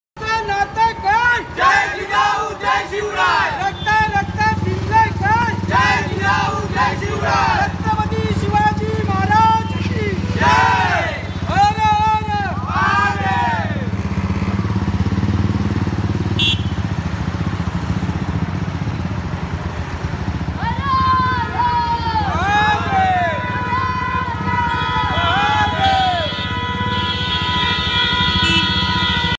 छत्रपति शिवाजी महाराज जयंती पर नालासोपारा में निकली भव्य बाइक रैली
12 मार्च को मावला प्रतिष्ठान महाराष्ट्र राज्य की ओर से रमाकांत बने के नेतृत्व में अयोजित छत्रपति शिवाजी महाराज जयंती पर भव्य बाकी रैली का आयोजन किया गया। इस मौके पर बाइक रैली सहित हल्दी कुमकुम सहित कई कार्यक्रम आयोजित किये गए, जिसमें स्थानीय लोगों ने बढ़-चढ़कर हिस्सा लिया।
- रवि जैन की रिपोर्ट